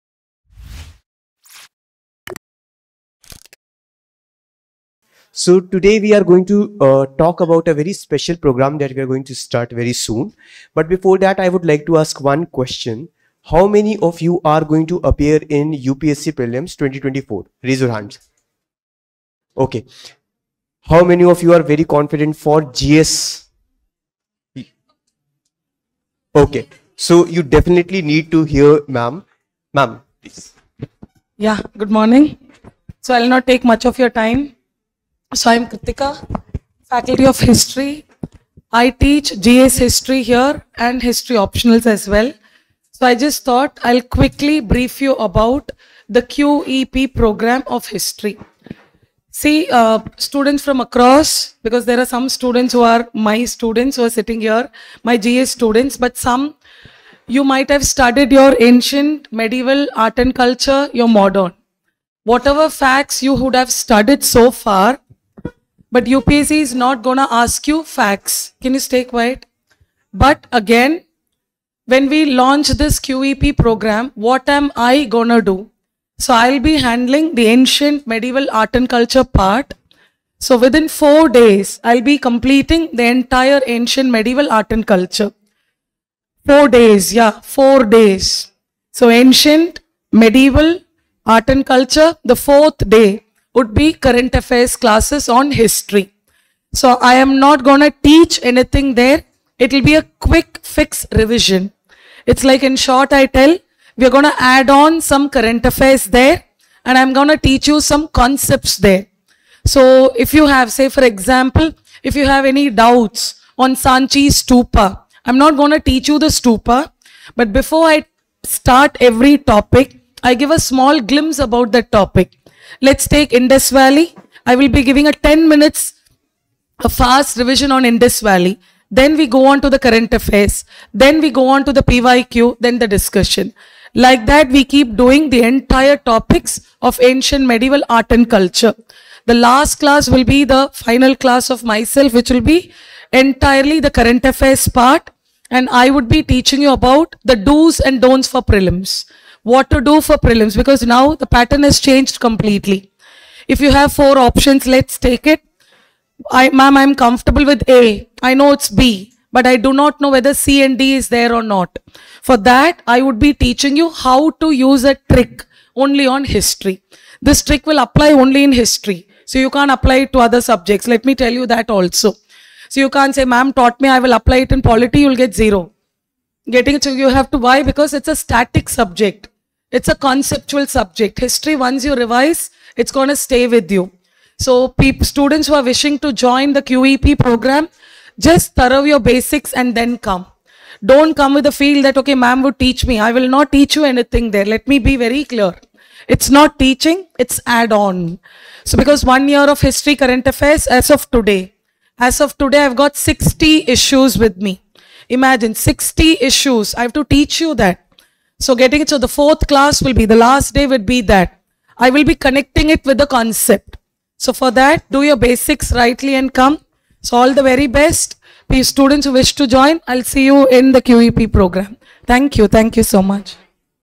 Quality Enrichment Program (QEP) | GS Prelims Crash Course | Batch Begins 19th Feb 2024
Prelims Quality Enrichment Program (QEP) - Crash Course for CSE Exam 2024 (Classroom Batch)

Prelims Quality Enrichment Program (QEP) Features:

A. Daily Classes:
- 45+ classes to cover most relevant topics (Static + Current Affairs) for Prelims.
- Cover one subject in one week (05 Hours 30 minutes /Class)

B. Workbooks/Handouts:
- Crisp, Precise, and high-quality handouts/Workbooks covering relevant points for Prelims.
- Help students with last-minute revision.

C. Current Affairs:
- Current Affairs Will Include Comprehensive Coverage Of India Year Book, Economic Survey, Budget & All Important Current Events.
- Comprehensively cover the last 1 year's relevant current affairs for Prelims
- All Standard sources such TheHindu, Indian Express, PIB, Budge, Economic Survey, and India Yearbook will be covered

D. Mentorship:
- One to One Doubt Clearing and Mentoring.
- Strategy Sessions will be taken by Faculties.

E. Prelims Test Series:
- Student will have access to 10 Sectional + 8 Full-Length Prelims Tests.

Class Timings : 8 AM to 01:30 PM
Classes : Monday to Saturday

Course Fee : Rs 15000/- (New Students) / Rs 8000 (Old Students)